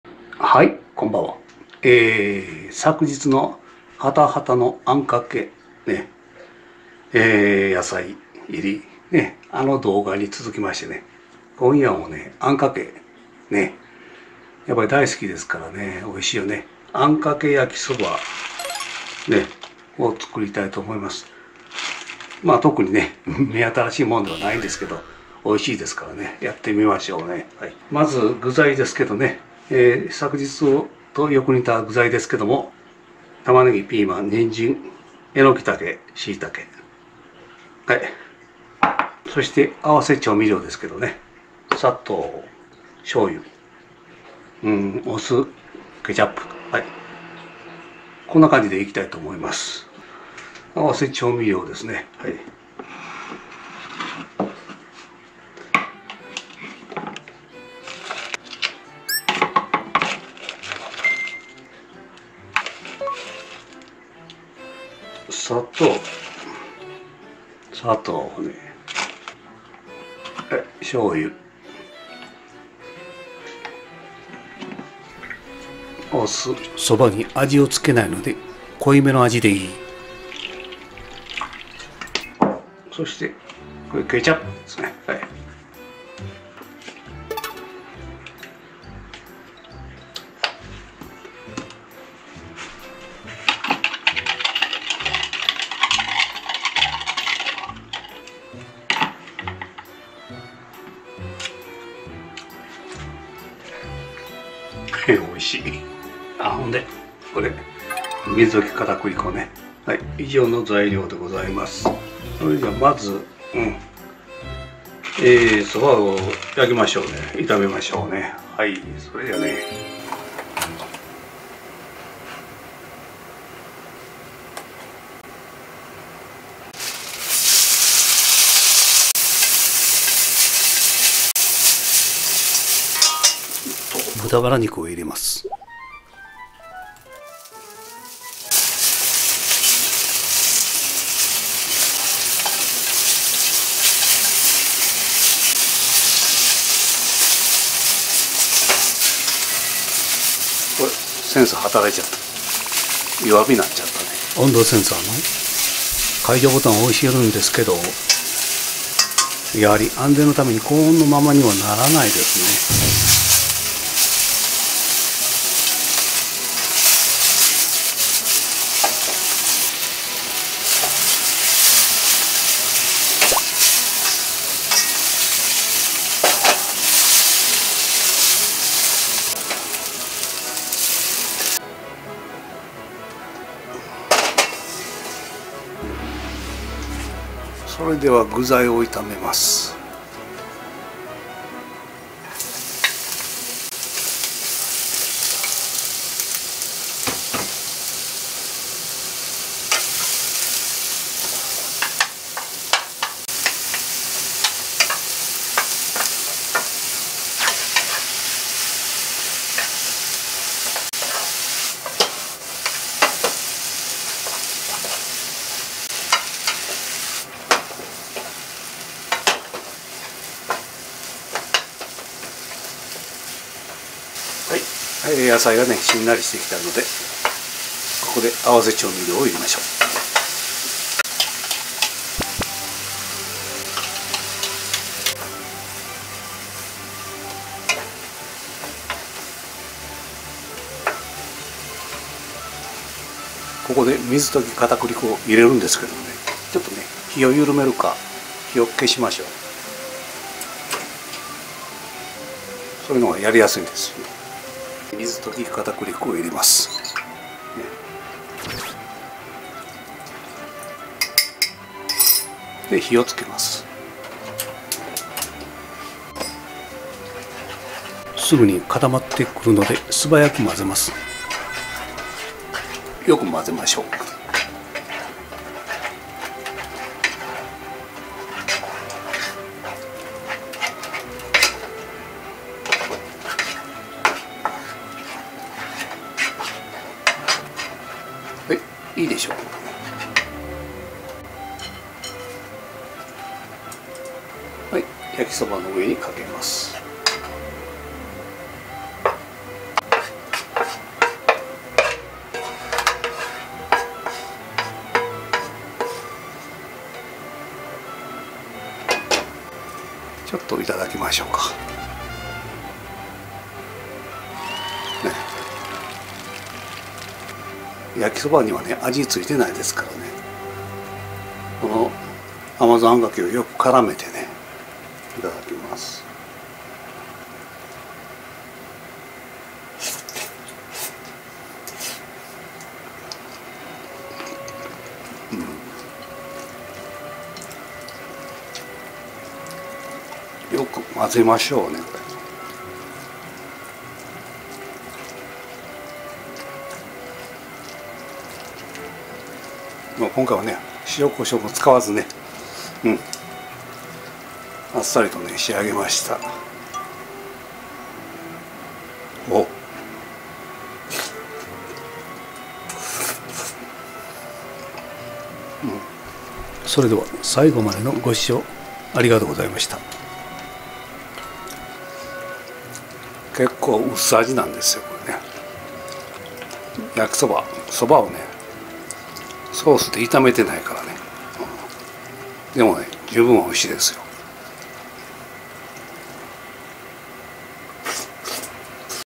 甘酢あんかけ！焼きそば☆合わせ調味料は！砂糖、お酢、しょう油、ケチャップで
昨日の ハタハタの「甘酢あんかけ」が美味しくて
今夜も、甘酢あんかけを、焼きそばに かけて「甘酢あんかけ焼きそば」に
今回は、そば と 豚バラ肉には、味をつけなかったので
結構アッサリとした、甘酢あんかけ焼きそばに！なりました。